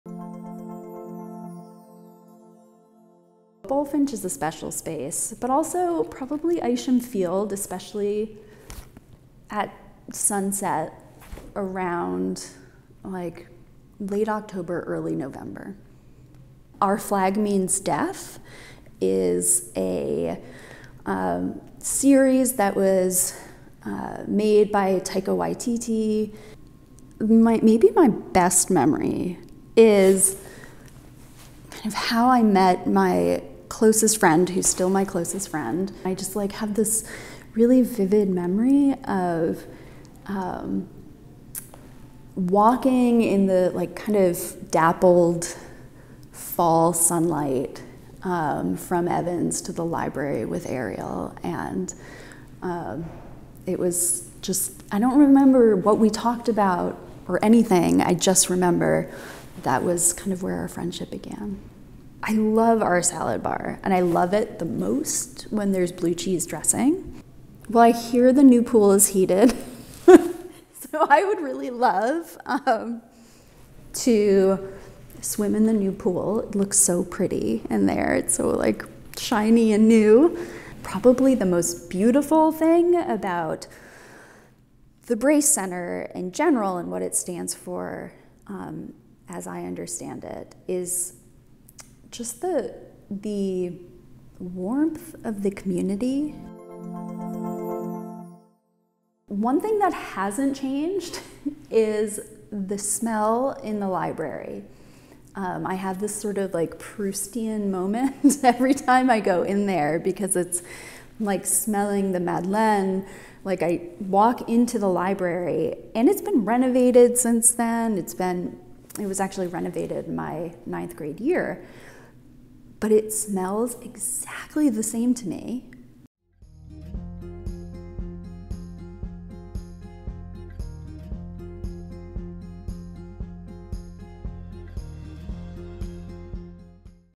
A Minute And Over: Patricia Har
📣 Meet Patricia Har ’95, our amazing English instructor and interim director of the Brace Center! As an alumna from the great class of 1995, she has a deep love and appreciation for Andover. We caught up with Ms. Har and asked her to share some of her favorite things about Andover, including what hasn't changed since she was a student. 🤔